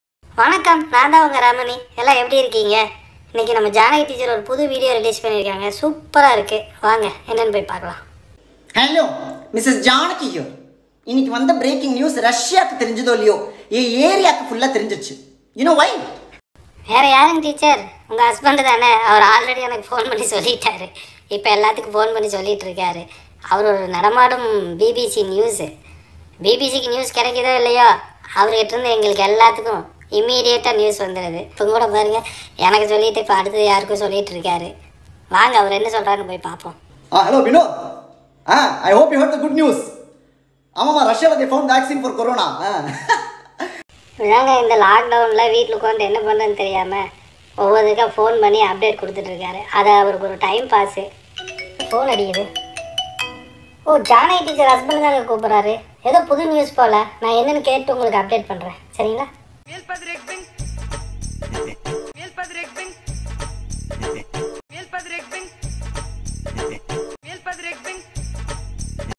Mrs.Janaki Reacts| Janaki Teacher comedy - corona vaccine Janaki Teacher Ramani - Fun Edits
World’s first corona vaccine is ready in russia and Mrs.Janaki’s husband can not control his excitement. Mr.Ramani Reacts. Watch, enjoy and share funedits !